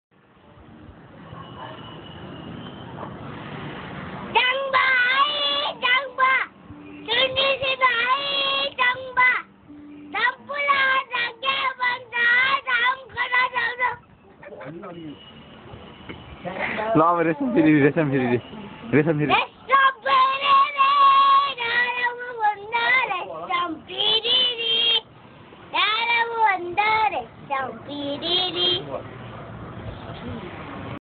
Kalden Sings
my cousin bro sings a nepaLi song.